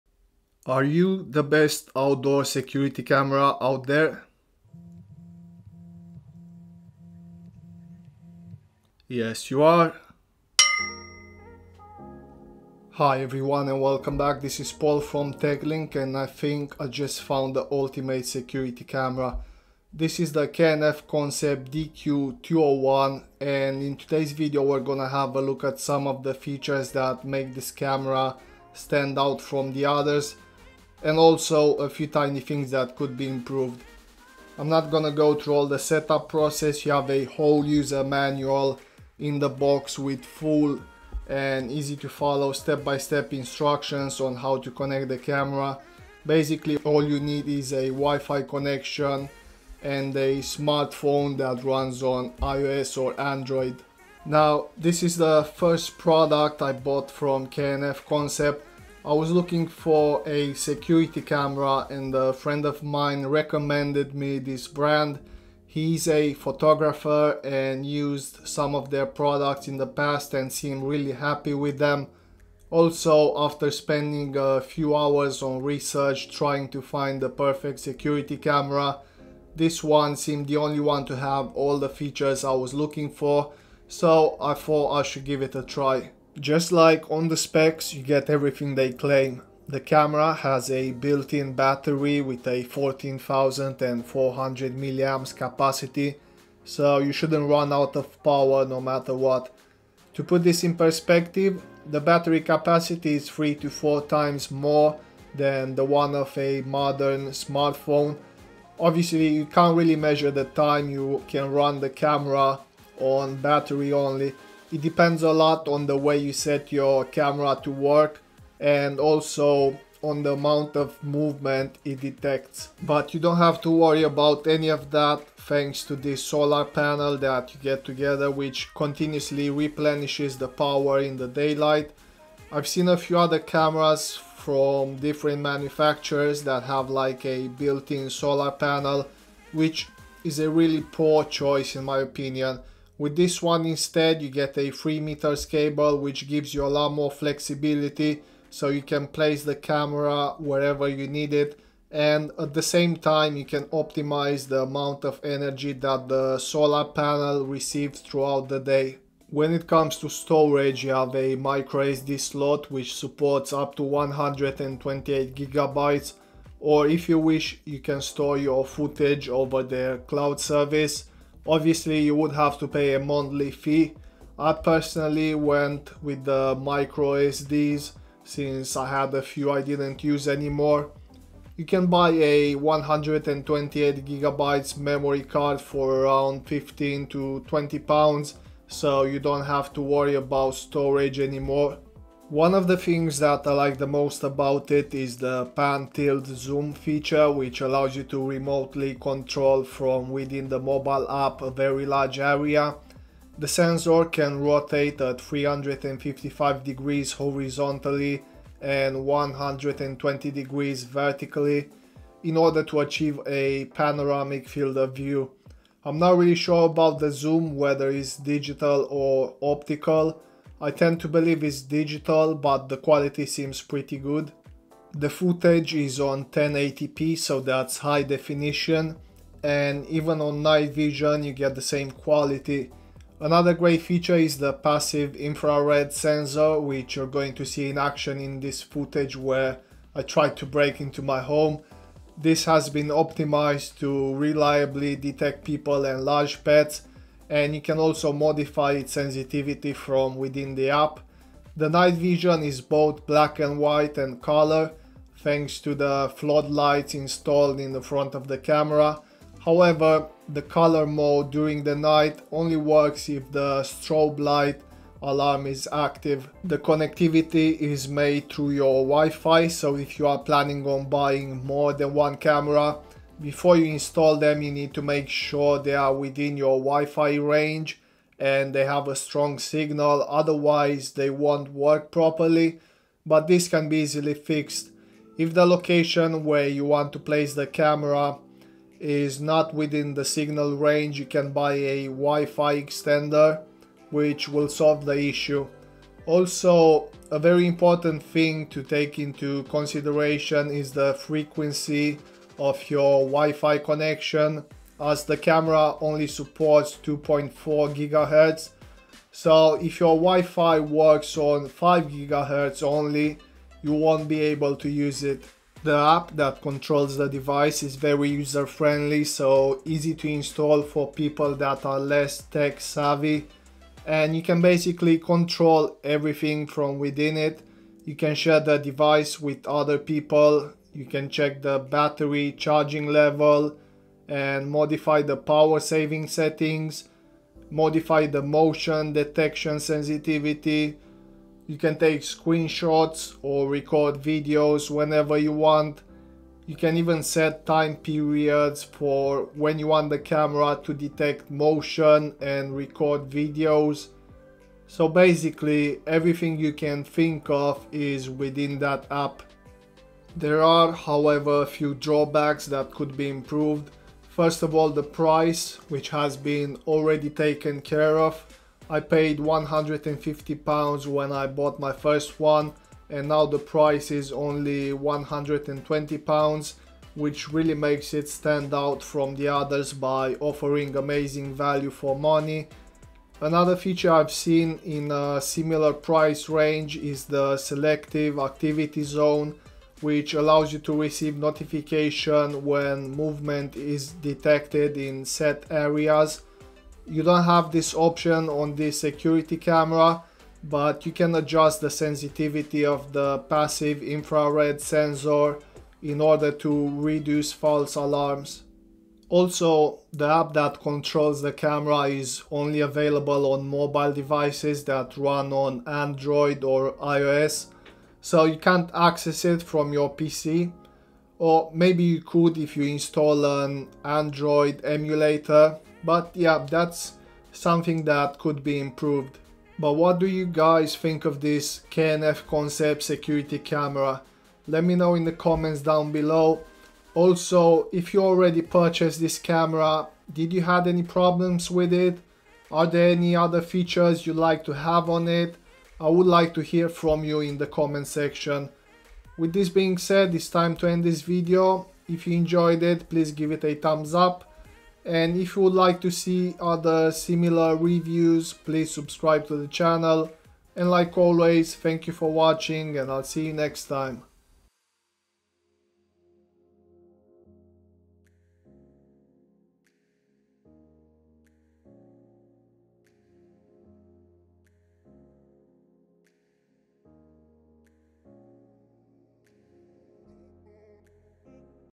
K&F Concept DQ201 Solar Security Camera Review ¬ Ultimate Security Camera on a BUDGET?
This is the K&F Concept DQ201 and in this video we're gonna have a look at some of the features that make this camera stand out from the others and also a few tiny things that could be improved. I'm not gonna go through all the setup process. You have a whole user manual in the box, with full, easy to follow, step by step instructions on how to connect the camera. Basically, all you need is a WIFI connection and a smartphone that runs on iOS or Android.

Home security is extremely important for each one of us and as the smart home tech continuously evolves day after day, so is the home security automation. There are so many choices out there when it comes to home security cameras that you might feel overwhelmed so in todays video were gonna have a look at what I retain to be the ultimate outdoor home security camera on a budget. I actually spent hours on research trying to find the perfect security camera and this one seemed the only one to have all the features I was looking for.

Some of the top features found on the K&F Concept DQ 201 Solar Security Camera are:
-100% Wireless & Solar Powered
-1080P Full HD & Colour Night Vision
-PIR Motion Detection & Sound-Light Alarm
-360°View & 2-way Audio
-Data Storage & After-sales Service

Time Stamp
0:00 Intro
0:54 The reason why I bought the K&F Concept DQ 201
1:26 Battery capacity
2:39 Different types of storage available (local & cloud)
3:12 The sensor capacities
4:26 Connect the camera to your Wifi network
5:15 The app that controls the device
6:00 Some drawbacks that could be improved
7:09 Outro

Hope you enjoyed it and if you did, I would like to hear from you in the comment section!

Support TechLink on Amazon